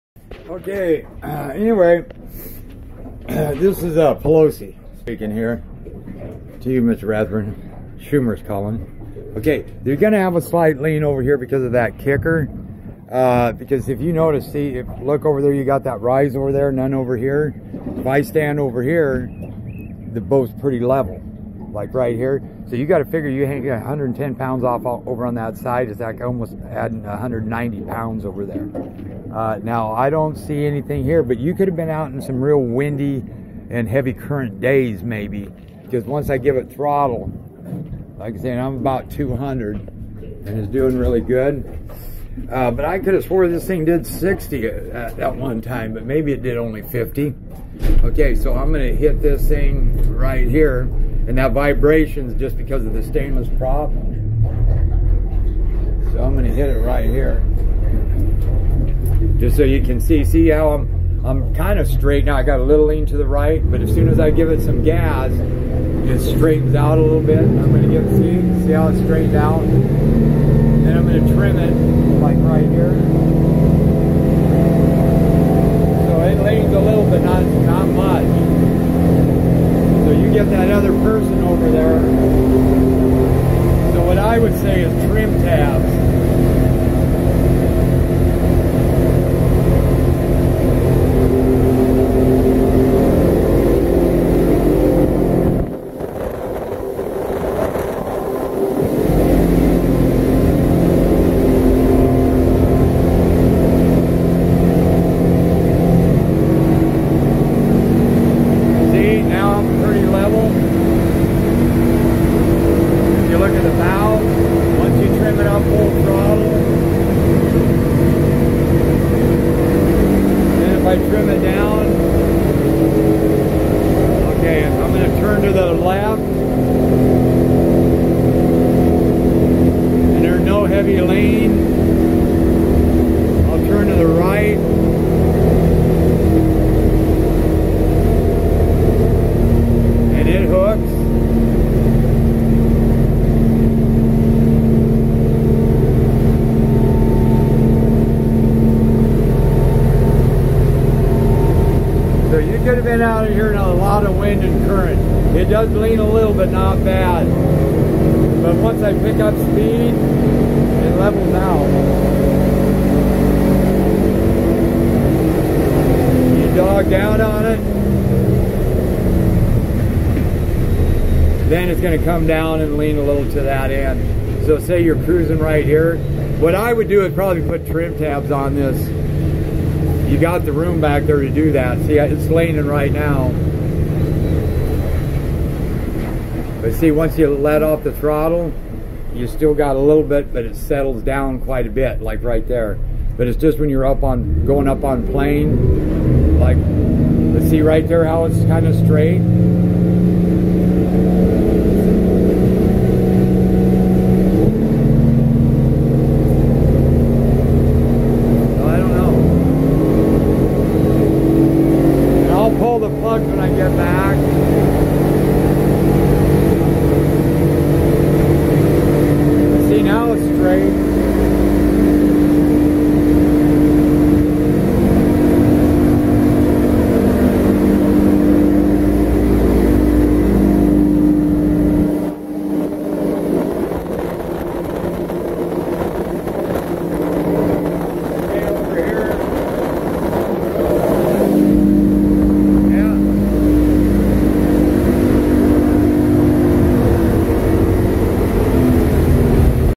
Rathburn 22 stabi craft lifting issue
runs great could use trim tabs if you have alot of weight to starboard side